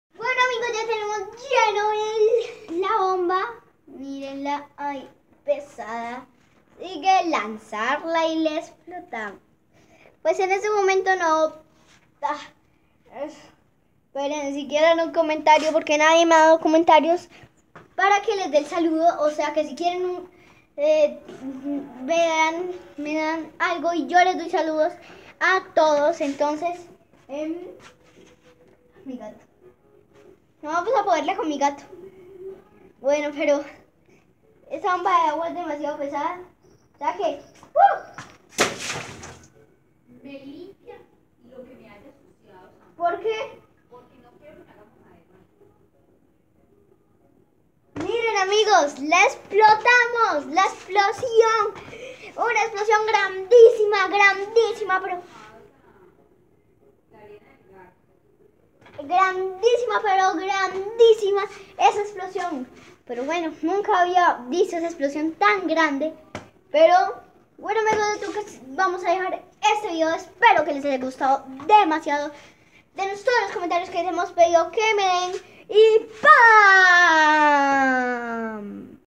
Mi gato explosión de agua
Disfruta de un video divertido donde mi gato salta¡¡¡